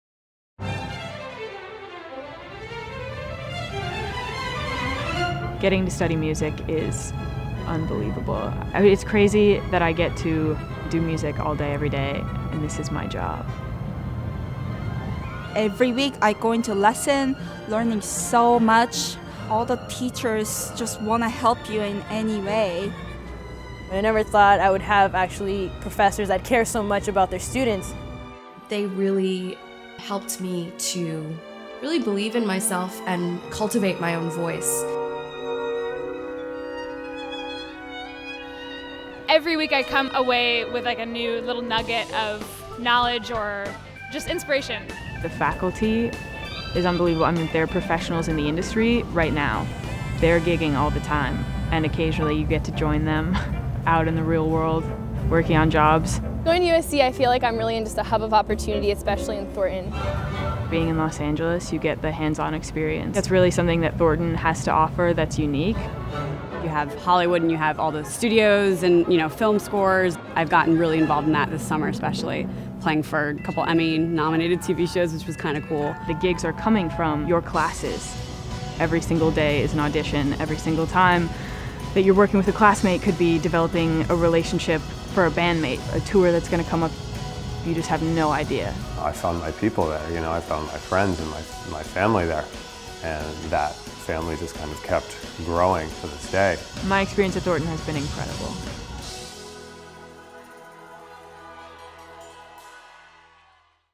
USC Thornton Students: In Their Own Words
What makes Thornton great? Don't take our word for it, let our students tell you what puts the USC Thornton School of Music on the map, from an extraordinary faculty to an unprecedented professional landscape. ✌️